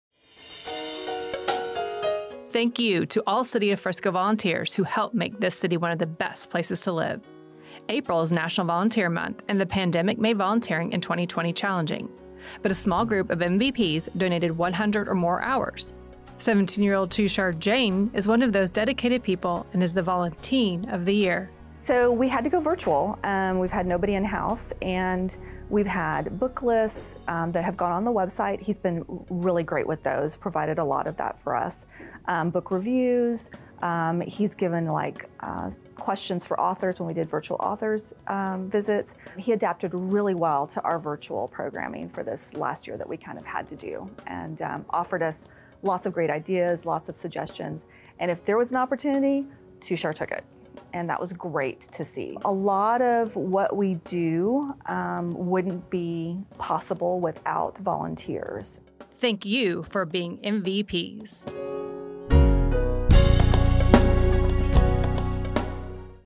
Thank You City of Frisco VolunTEEN of the year
April is National Volunteer Month.  17-year-old Tushar Jain is a Junior at Frisco Centennial HS.  During 2020, he contributed 126 hours, all virtually, to the Frisco Public Library and he’s the VolunTEEN of the year!  Thank you Tushar for being a MVP!